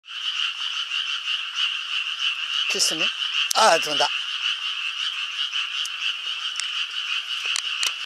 DSCF8027蛍
東北町のホタルの里にてホタル観察・仲間同士でこの里を作りあげている。